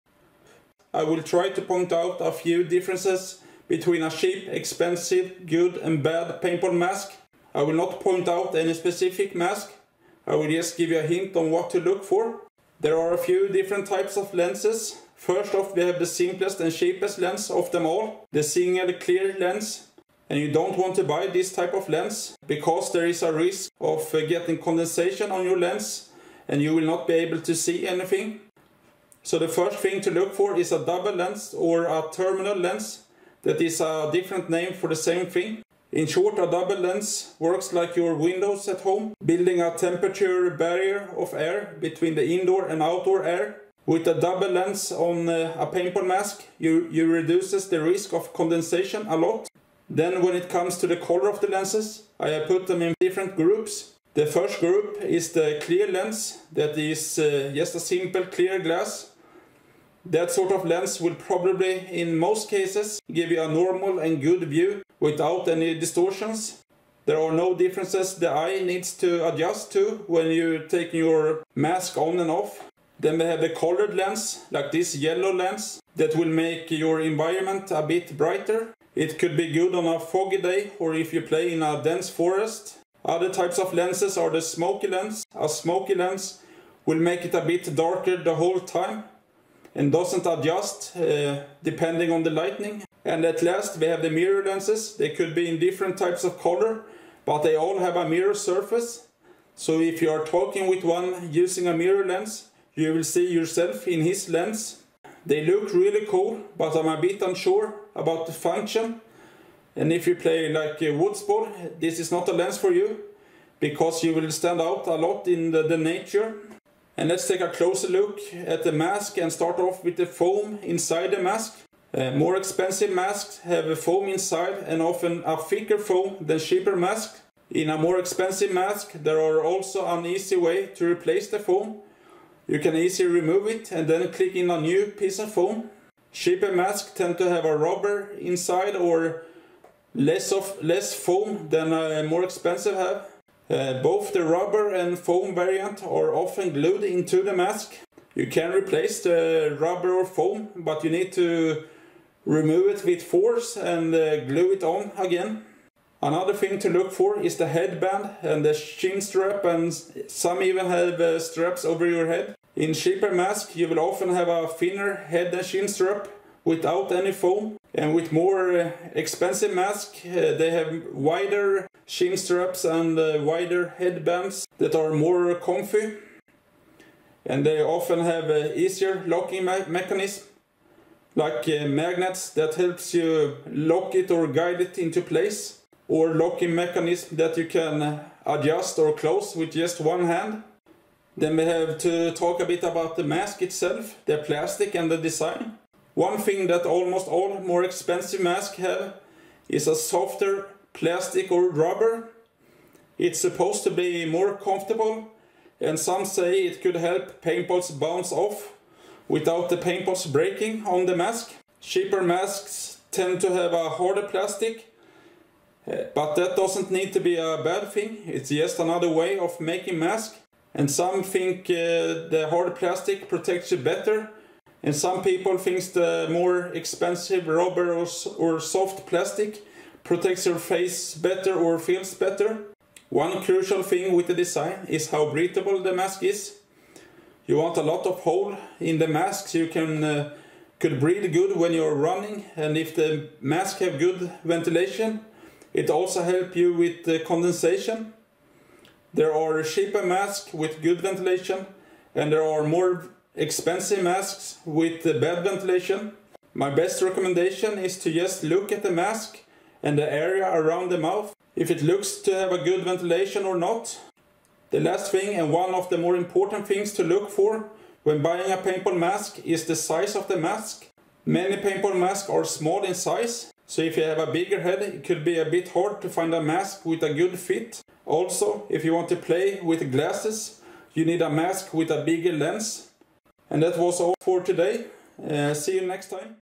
5 min quick guide to buying your first paintball mask
5 min quick guide about paintball masks and what to look for when buying a new mask.
Short information about lenses, straps, design, and so on.